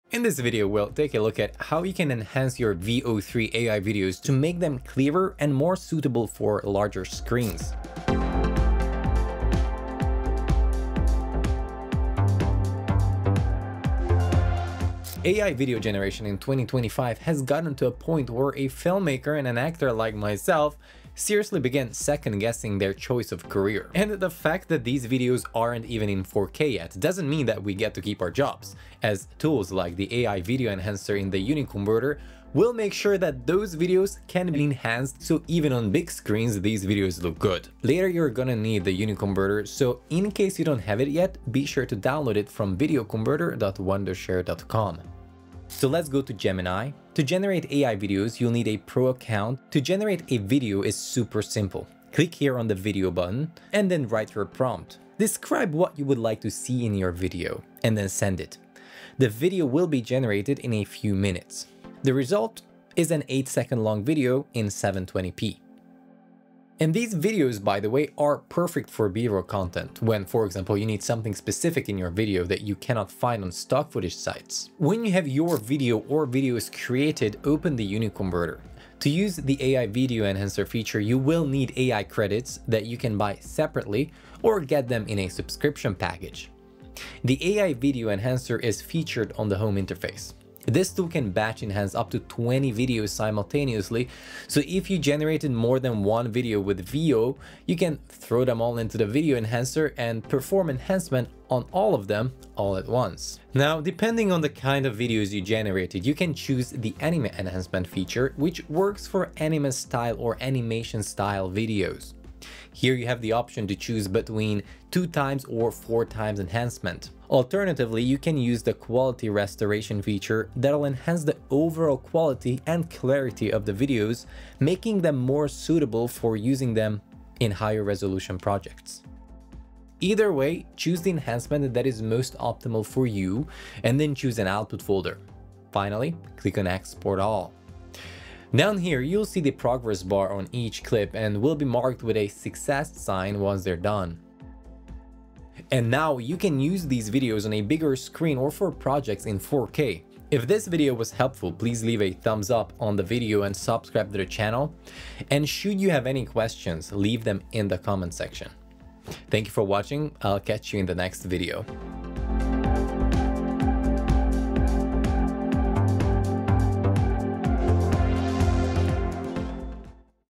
Upscale VEO3 AI Videos to 4K with UniConverter | Fast & Easy Tutorial
🎬 Creating AI videos with VEO3 but frustrated they're only 720p? No worries — in this video, I’ll show you how to enhance your VEO3-generated AI videos to stunning 4K resolution using Wondershare UniConverter’s AI Video Enhancer. Try it now

🧠 Perfect for content creators, marketers, and AI video enthusiasts who want their clips to look crisp and professional even on big screens.

📌 What you'll learn:
How to generate short videos with VEO3 AI
How to use UniConverter to batch upscale to 4K
The difference between Anime-style vs. Quality Restoration enhancement
Export tips for high-resolution output

🛠️ Tools used:
VEO3 AI Video Generator
Wondershare UniConverter

⌚ Timestamps
00:00 - Why VEO3 AI Videos Need 4K Enhancement
00:35 - How to Generate AI Videos with VEO3
01:25 - Batch Upscaling to 4K with UniConverter
02:15 - Export Settings and Final Output Tips